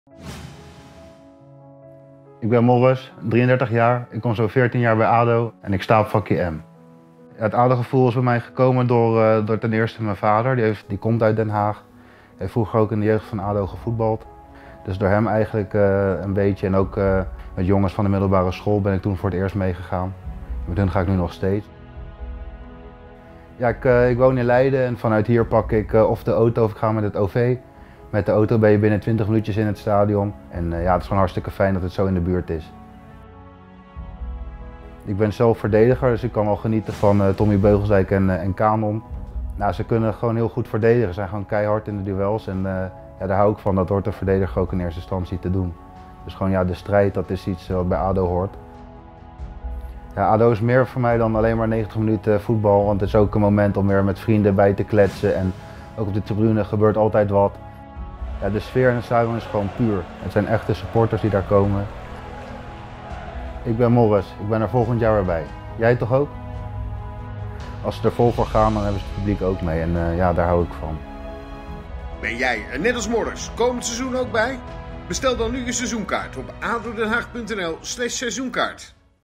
"Naast de 90 minuten voetbal ook een moment om bij te kletsen met vrienden", aldus Morris (vak M)
Bestel nu je Seizoenkaart en beleef onze wedstrijden al vanaf €9,- per wedstrijd!